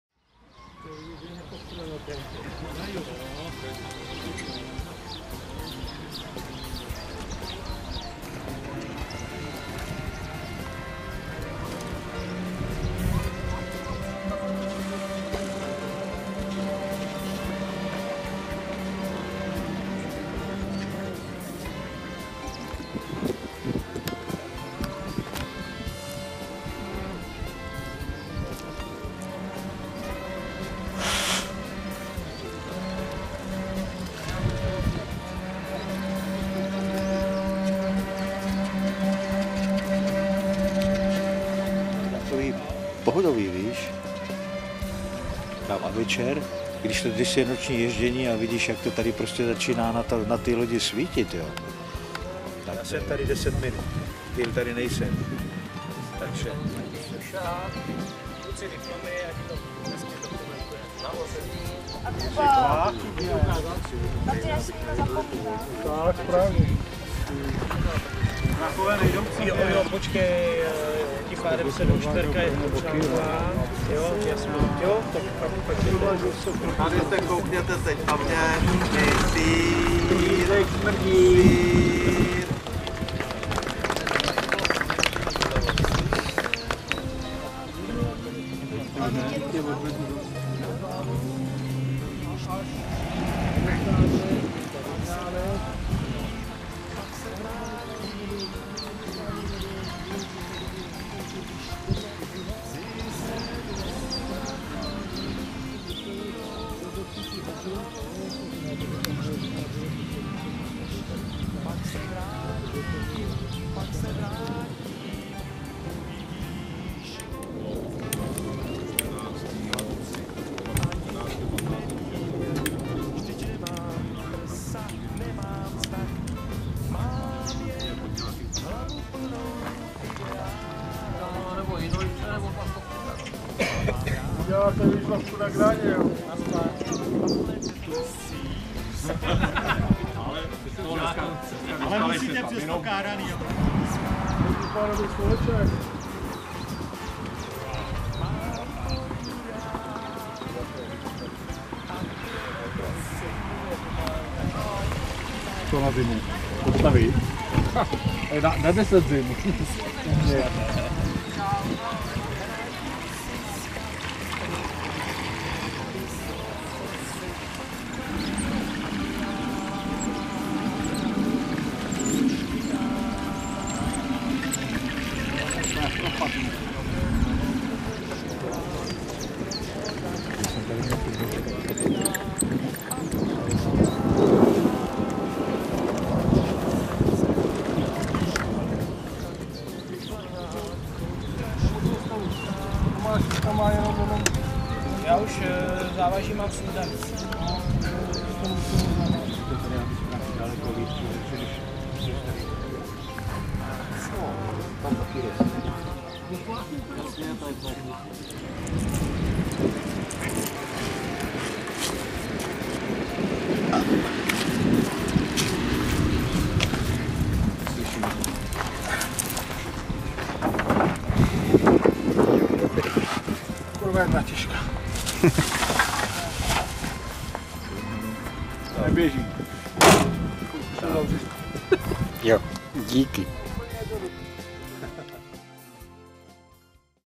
Nová ves duben 2019
Protože jsme přijeli pozdě, vlastní soutěž modelářů jsme neviděli. Závěr 1. akce v roce 2019 na rybníku v Nové vsi a vyhlášení výsledků, včetně úklidu tratě jsme stihli.